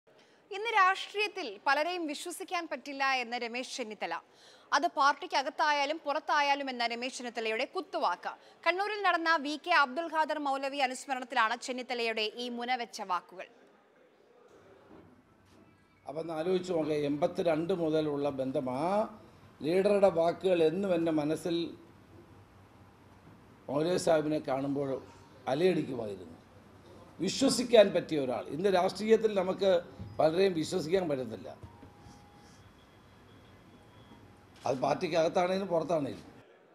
ഇന്ന് രാഷ്ട്രീയത്തിൽ പലരേയും വിശ്വസിക്കാൻ പറ്റില്ലെന്ന് രമേശ് ചെന്നിത്തല| Mathrubhumi News
ഇന്ന് രാഷ്ട്രീയത്തിൽ പലരേയും വിശ്വസിക്കാൻ പറ്റില്ലെന്ന് രമേശ് ചെന്നിത്തല. അത് പാർട്ടിക്ക് അകത്തായാലും പുറത്തായാലുമെന്നും രമേശ് ചെന്നിത്തലയുടെ കുത്തുവാക്ക്.

Happy viewing!